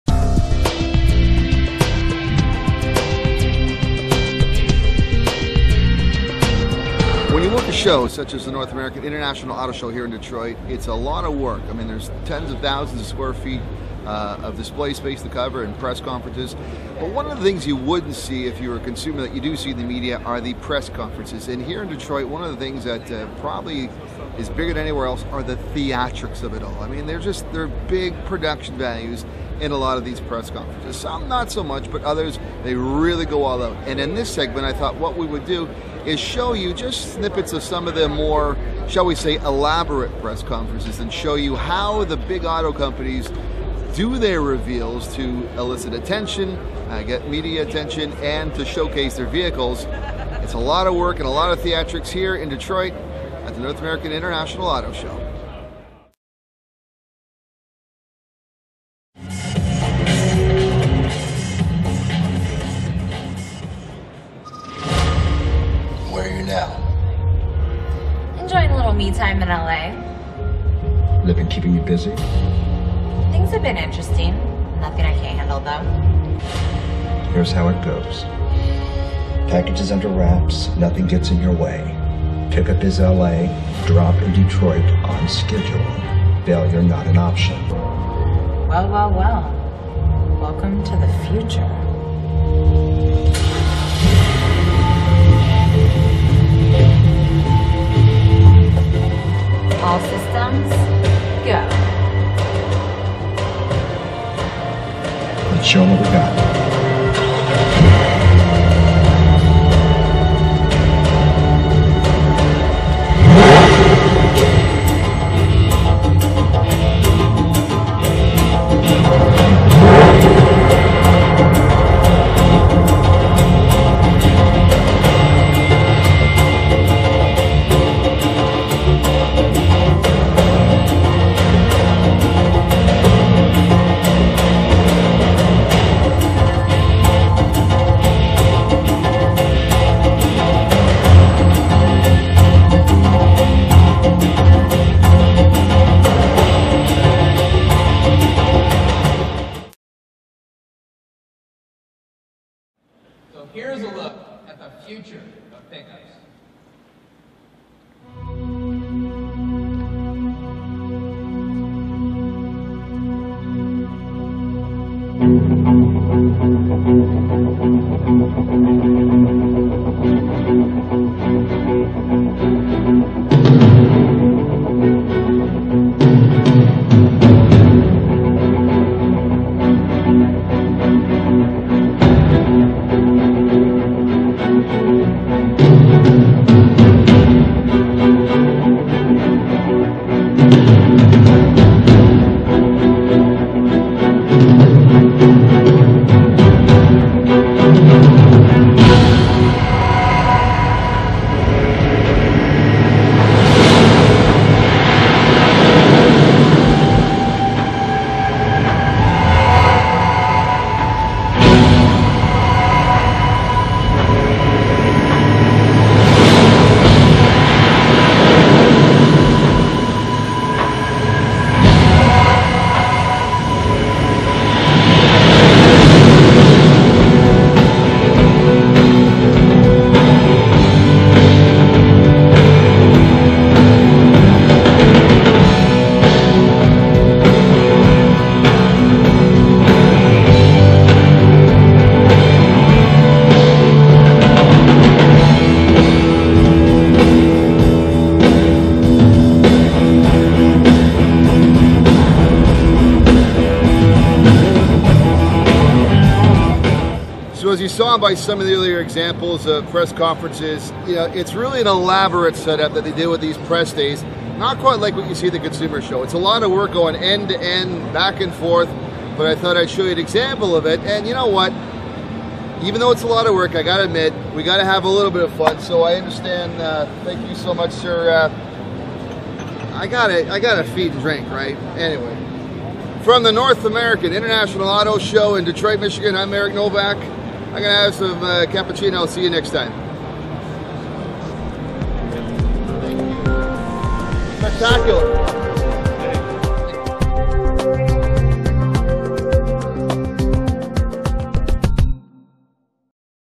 Spectacular Press Conferences at NAIAS 2013
The North American International Auto Show in Detroit is often where many car manufacturers will launch new vehicles, and part of the launch process are often spectacular Press Conferences for the 6,000 accredited media on hand.  This report showcases two of the more elaborate launches held by Hyundai and Ford.